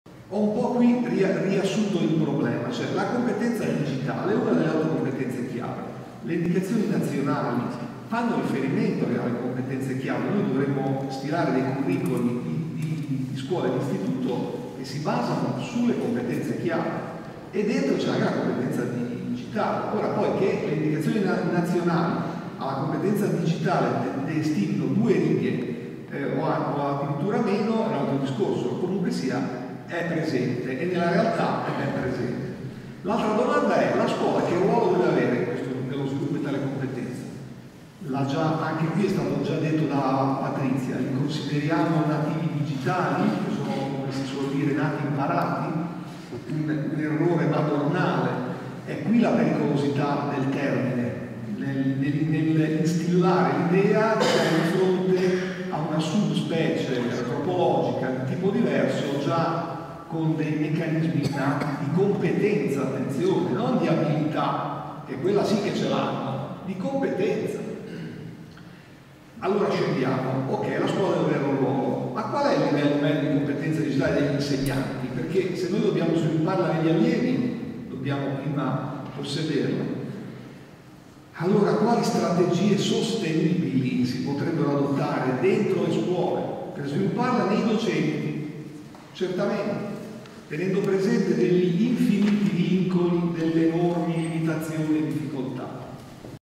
Antonio Fini - estratto n. 1 della relazione "Scenari “sostenibili” per le tecnologie scolastiche"
"Come le tecnologie digitali possono sostenere processi di costruzione di significato a scuola. Scenari “sostenibili” per le tecnologie scolastiche". Relazione di Antonio Fini al seminario "Resistenza e seconda guerra mondiale", secondo corso di formazione INSMLI sull’insegnamento della storia nell’era digitale.

Un sunto della relazione è pubblicato nel dossier "Resistenza e seconda guerra mondiale", inserito nel numero 3 della rivista di didattica della storia Novecento.org